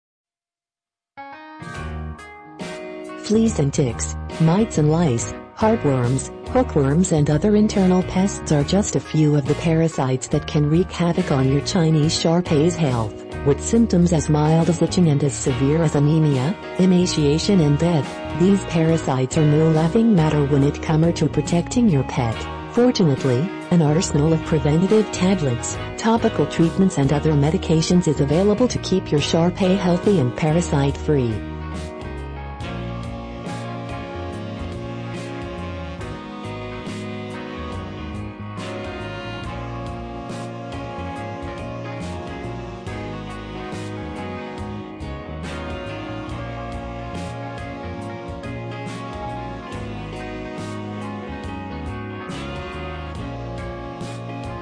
Chinese Shar-Pei Parasite Prevention
Protect Your Chinese Shar-Pei from all parasites like Flea, Tick, Heartworm and Intestinal Worms.